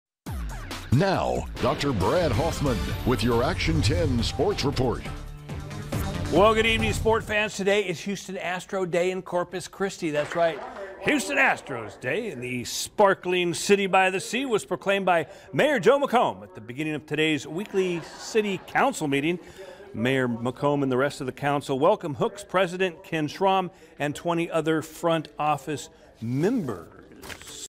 'Houston Astros Day' proclaimed in Corpus Christi
Corpus Christi has a new special holiday to celebrate as of today thanks to the 2017 World Series Champions.